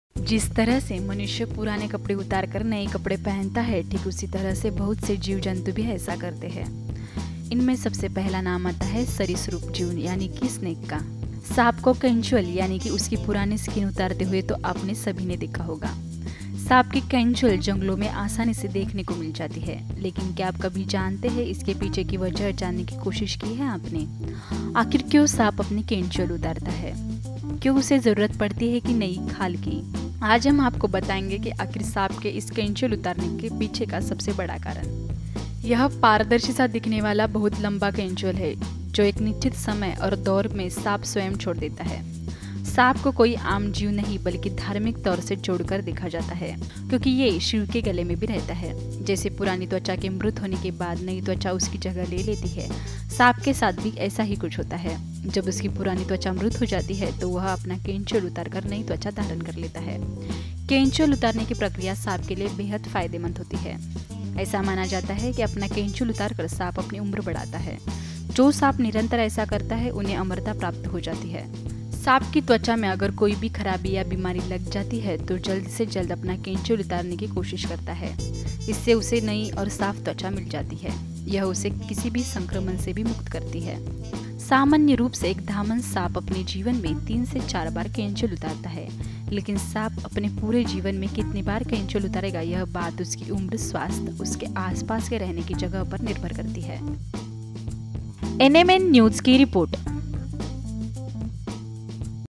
सांप क्यों उतारता हैं अपनी केंचुली?
जिस तरह से मनुष्य पुराने कपड़े उतारकर नए कपड़े पहनता है ठीक उसी तरह बहुत से जीव-जंतु भी ऐसा करते हैं। इसमे सबसे पहला नाम आता है सरीसृप जीव यानि कि snake का। सांप को केंचुल यानि कि उसकी पुरानी स्किन उतारते हुए तो आप सबने देखा होगा। सांप की केंचुल जंगलों में आसानी से देखने को मिल जाती है। लेकिन क्या कभी आपने इसके पीछे की वजह जानने की कोशिश की है। आखिर क्यों सांप अपनी केंचुल उतारता है। क्यों उसे जरूरत पड़ती है नई खाल (केंचुल) की। आज हम हम आपको बताएंगे की आखिर सांप के इस केंचुल उतारने के पीछे का सबसे बड़ा कारण।